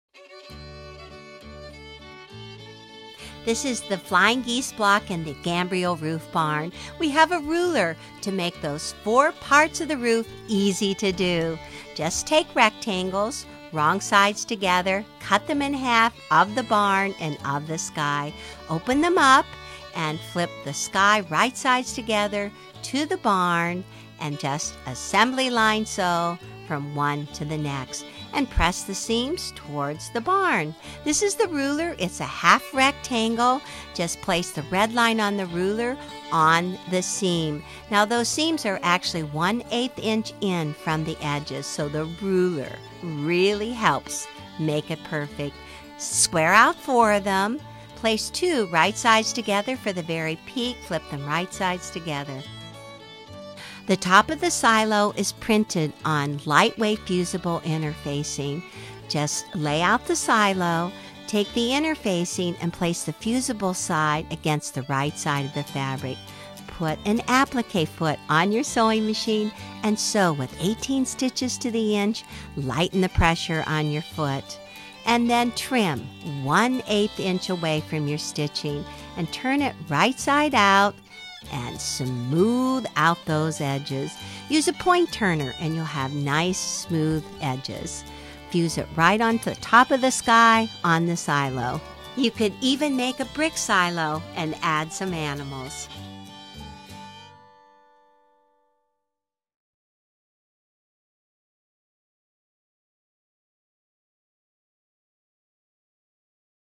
Eleanor Burns makes a Gambrel Roof Barn
Featuring the Flying Gees block.

Flying geese the easy way, the Quilt in a Day way.